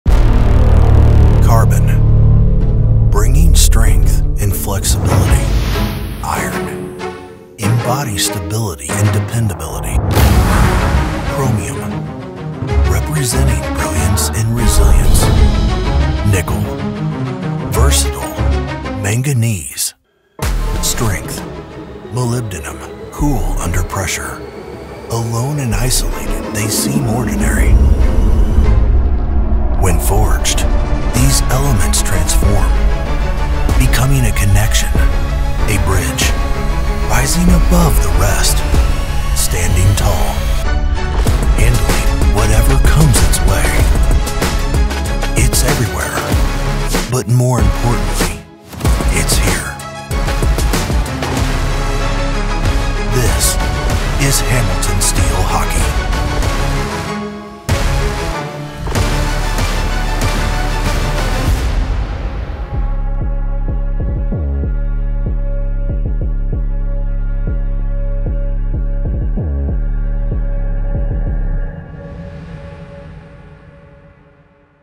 2023 Hamilton Steel OMHA OHF Jersey Reveal Top Shelf Media
The Hamilton Steel are here in hamont

The Hamilton Huskies AAA and Jr Bulldogs joined forces to form the Hamilton Steel Hockey Club (HamiltonSteelHockeyClub.com) and we got to capture one of the most exciting moments in AAA Hamilton hockey history as the Steel went all in on the Steel name.

The Steel brand grew overnight and was a success in the parents were commenting on how everything was on a professional level not seen normally at the AAA level which was just what we wanted.

Learn more about what we do at tsmedia.ca